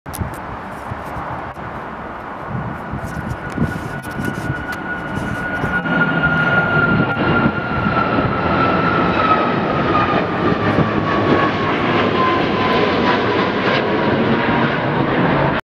Icelandair B737-9 TF-ICD Takeoff from London Gatwick
Aircraft Information

Airline  Icelandair

Aircraft  B737-9

Reg  TF-ICD

Engines 2x CFMI CFM Leap-1B

Destination London/Gatwick-Reykjavik

Route LGW-KEF